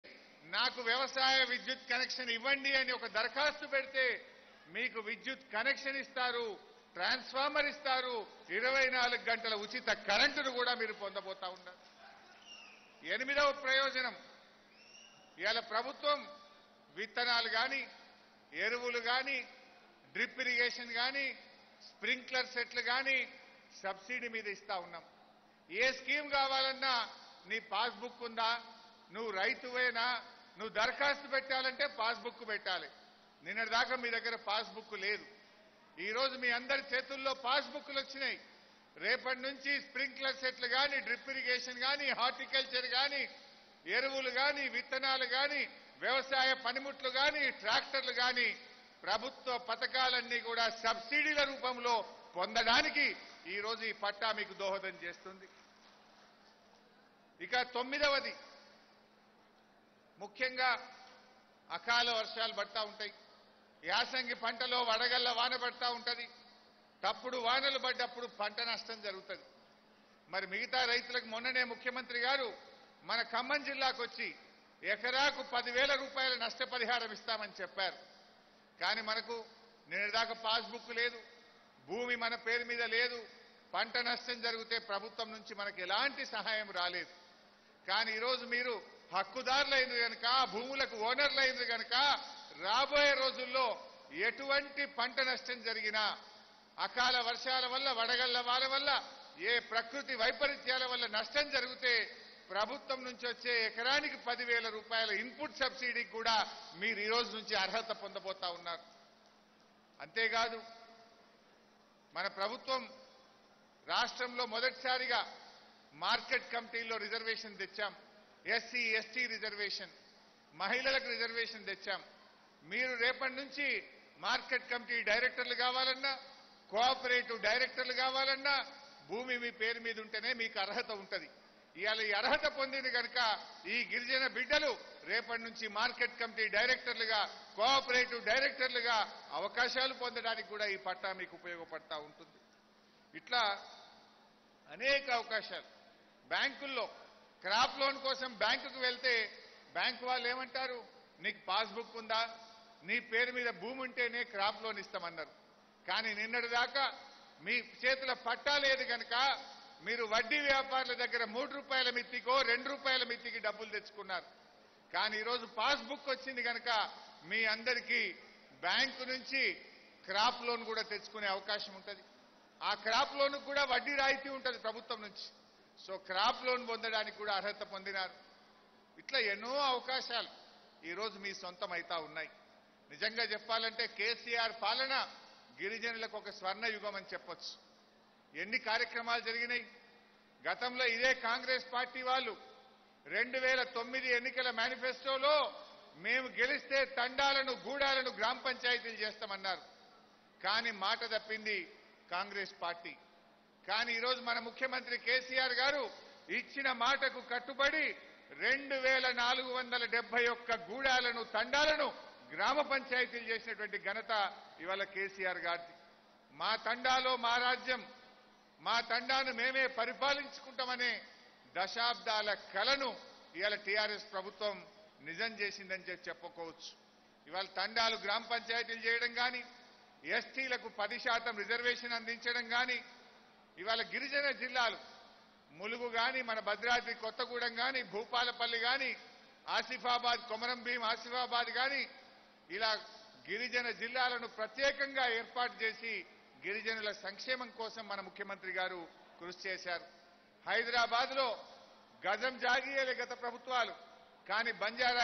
పోడు భూములకు రైతుబంధు , రాయితీలు .. హరీష్ రావు..| Harish Rao Distribution Of Podu Land Pattas
పోడు భూములకు రైతుబంధు , రాయితీలు .. హరీష్ రావు..| Harish Rao Distribution Of  Podu Land Pattas | Focus News Telugu